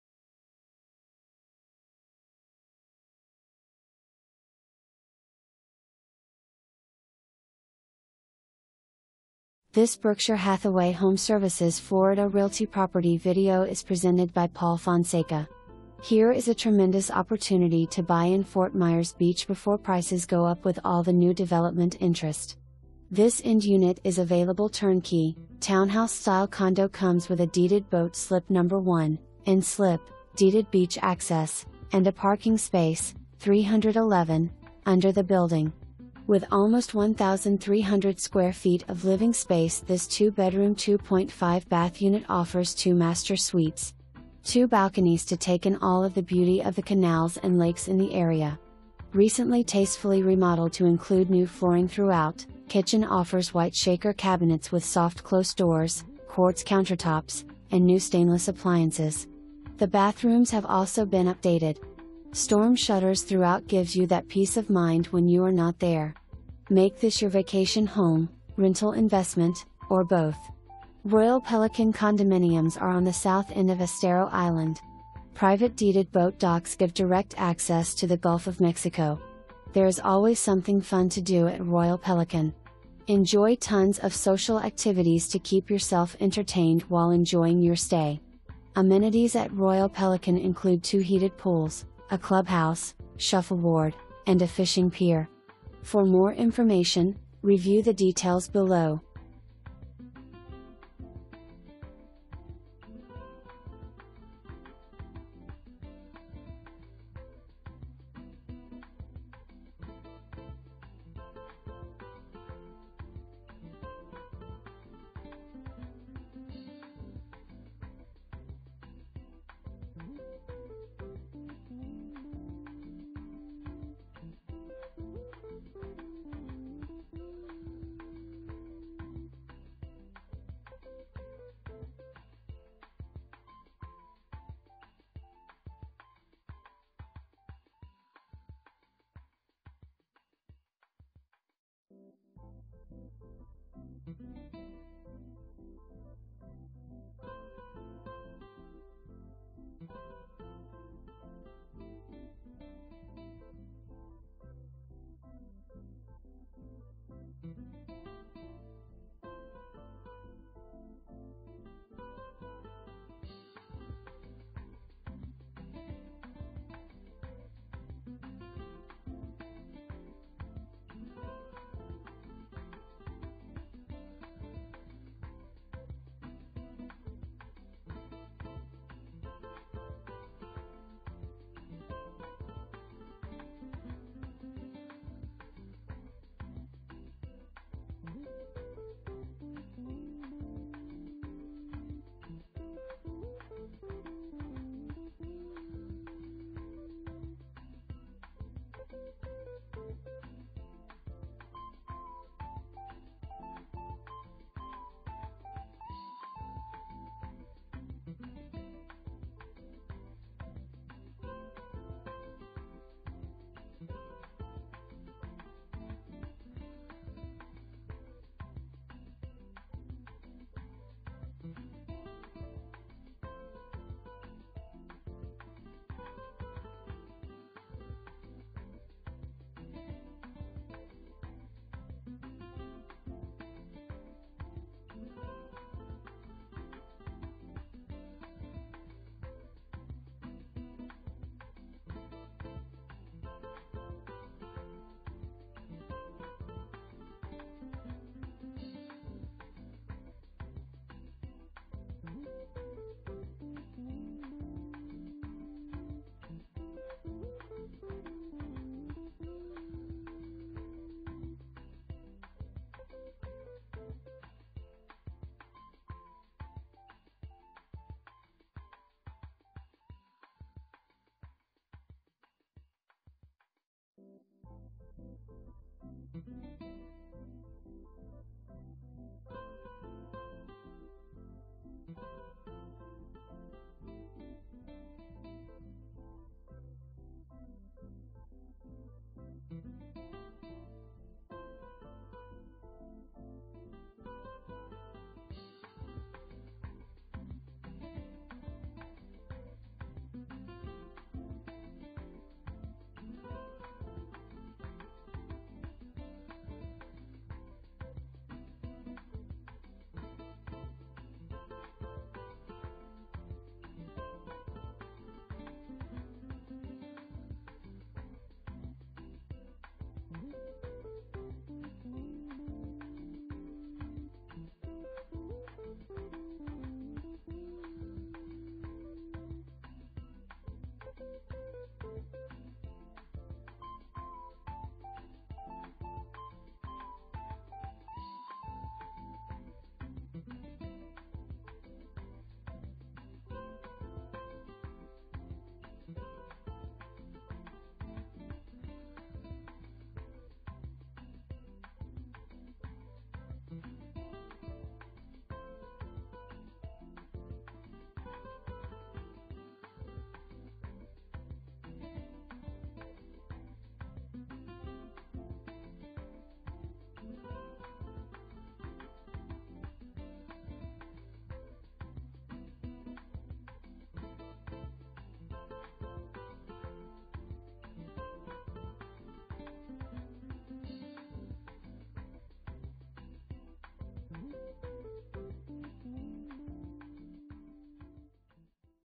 Berkshire Hathaway HomeServices Florida Realty - 4511 Bay Beach LN 311
HERE IS A TREMENDOUS OPPORTUNITY TO BUY IN FORT MYERS BEACH BEFORE PRICES GO UP WITH ALL THE NEW DEVELOPMENT INTEREST! This End Unit is available TURNKEY, Townhouse style condo comes with a Deeded Boat Slip #1 (End Slip), Deeded Beach Access, and a Parking Space (311) under the building. With almost 1300 square feet of living space this 2 bedroom 2.5 bath unit offers two master suites. 2 balconies to take in all of the beauty of the canals and lakes in the area. Recently tastefully Remodeled to include new flooring throughout, Kitchen offers white shaker cabinets with soft close doors, Quartz Countertops, and New Stainless Appliances. The bathrooms have also been updated. Storm Shutters Throughout gives you that piece of mind when you are not there. Make this your Vacation Home, Rental Investment, or Both! Royal Pelican Condominiums are on the south end of Estero Island. Private deeded boat docks give direct access to the Gulf of Mexico. There is always something fun to do at Royal Pelican! Enjoy tons of social activities to keep yourself entertained while enjoying your stay. Amenities at Royal Pelican include 2 heated pools, a clubhouse, shuffleboard, and a fishing pier.

Paul Fonseca
Berkshire Hathaway HomeServices Florida Realty